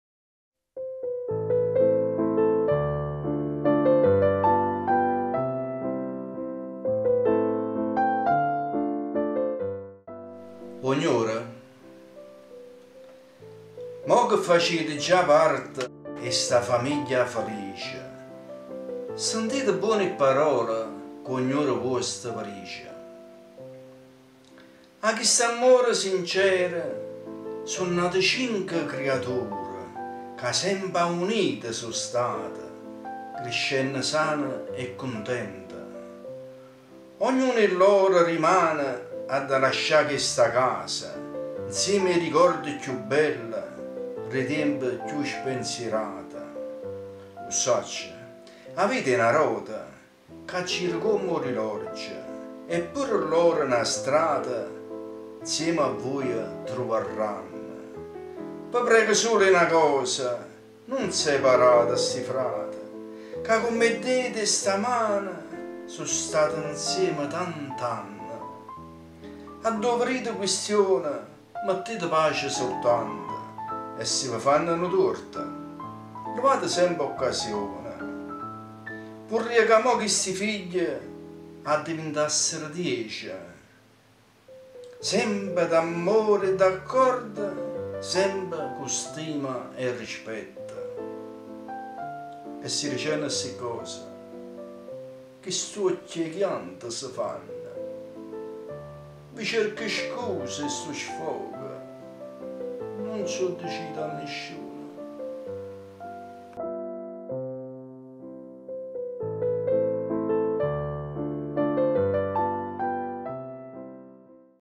'O gnore - Poesia - Bruno Iaccarino
Le nuore e i generi, dovrebbero
amare e essere amati, come se
fossero altri figli.